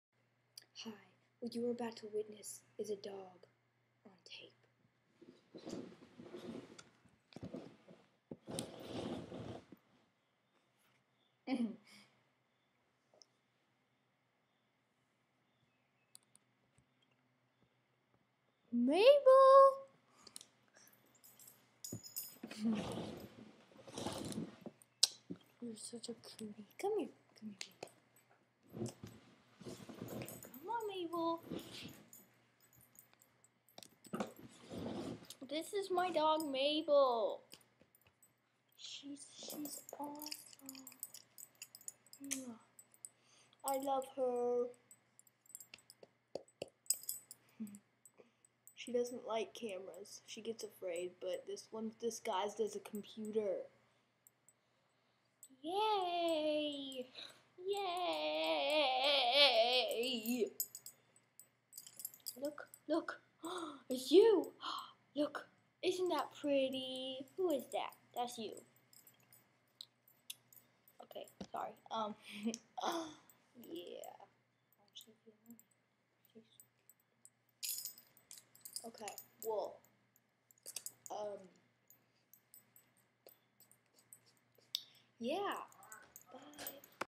Mable!!!!!
My awesome dog! [she is a Black & Tan Coonhound]